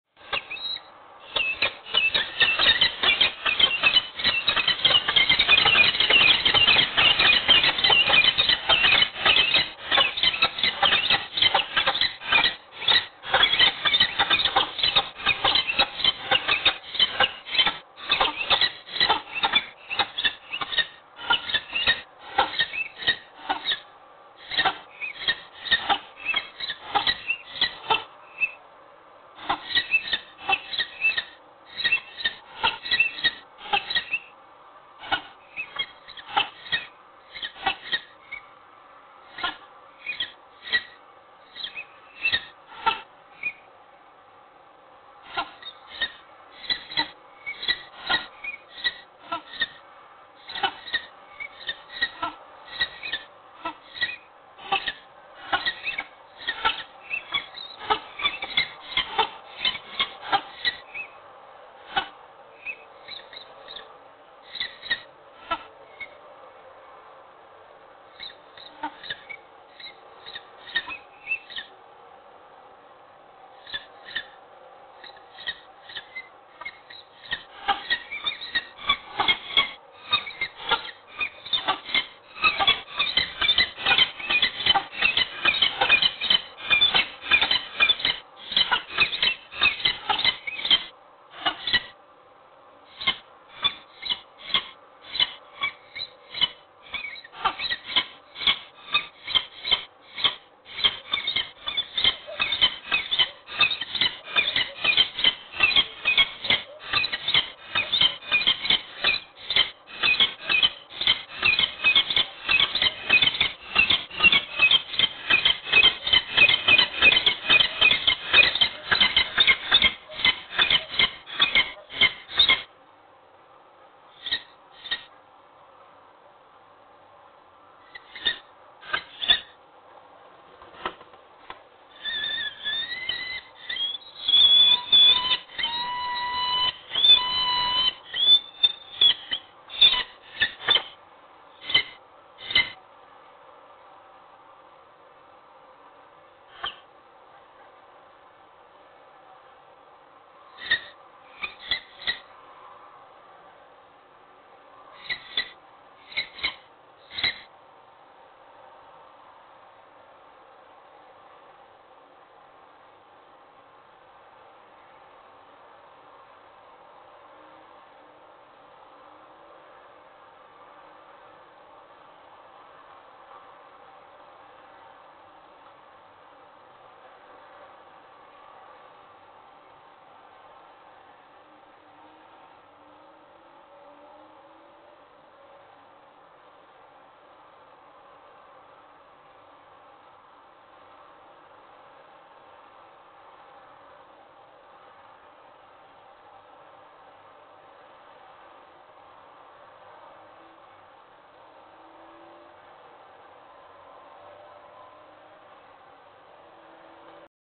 Sokół wędrowny-Wołomin
Poranne spotkanie i zalężenie.